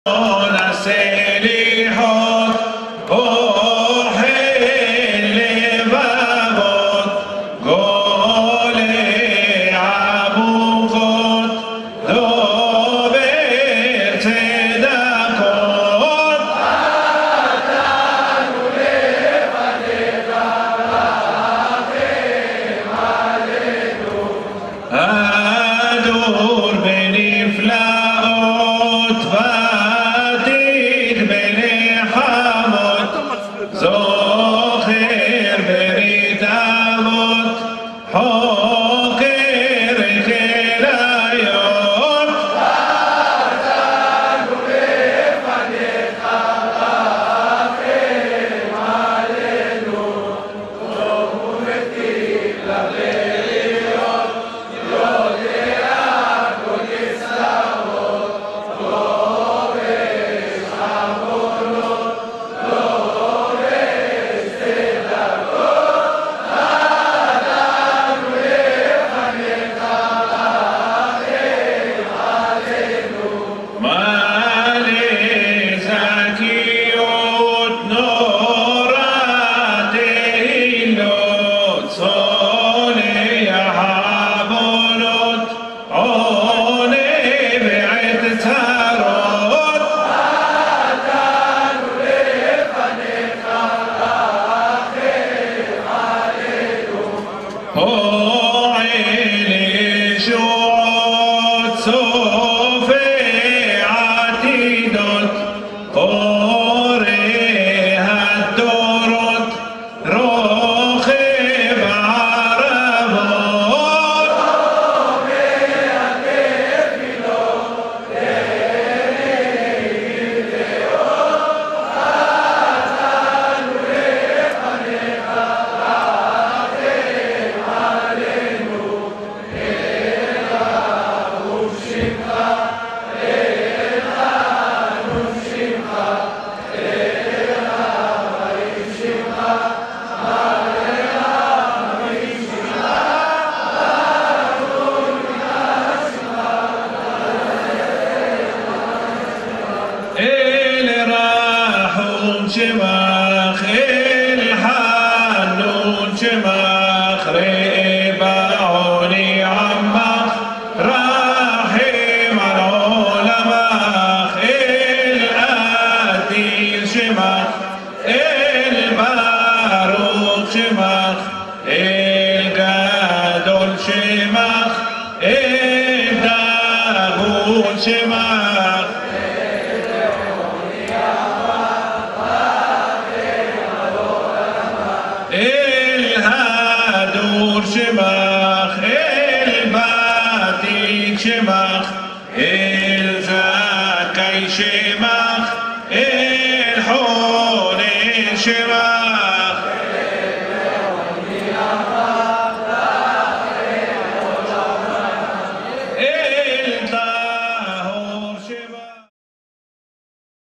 Emotional 100,000 Attend Selichot at the Western Wall
Tens of thousands of Jews visited the Western Wall (Kotel) in Jerusalem on Monday night, gathering to ask for forgiveness and mercy in Selichot prayers before the Yom Kippur (Day of Atonement) fast despite - or in spite of - the tense security situation in the capital.

As many as 100,000 people may have visited the Kotel throughout the night for the culmination of the penitentiary prayers, which ends some 40 days of prayer for Sephardic Jews and roughly two weeks for Ashkenazic Jews.

The prayer drew a variety of people - men and women, children and the elderly, and observant and non-observant Jews.
Both Ashkenazic Chief Rabbi David Lau and Sephardic Chief Rabbi Rabbi Yitzhak Yosef attended the service, as well as Kotel Rabbi Rabbi Shmuel Rabinowitz, attended the services.